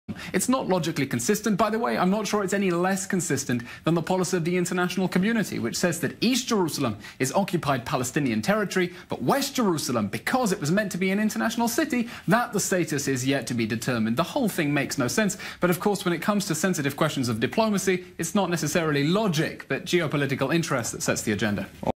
International policy on Jerusalem is incoherent
International policy on Jerusalem makes no logical sense: East Jerusalem is Occupied Palestinian Territory, but West Jerusalem's status is to be determined.